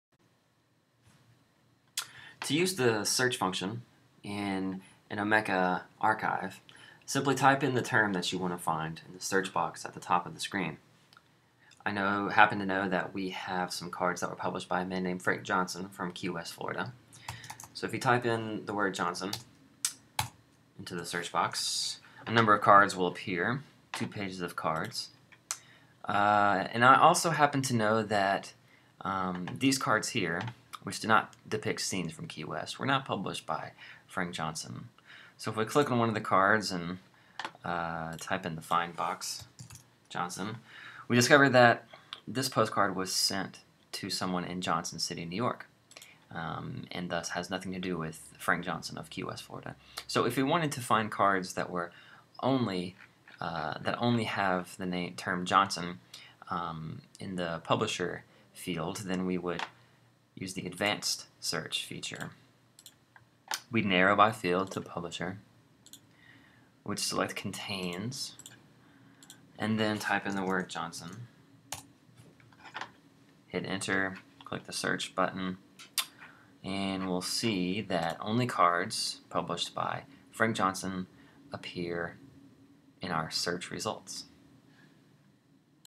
"Searching the FSU card archive" by Michael Neal et al. (Kairos 17.3 Topoi)
Here, a founder of the archive, Stephen J. McElroy walks users through a tutorial on how to use the archive's multiple search functions. This text is published in the May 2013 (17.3) issue of the Topoi section of Kairos: A Journal of Rhetoric, Technology, and Pedagogy. Kairos is a free, online, peer-reviewed scholarly journal available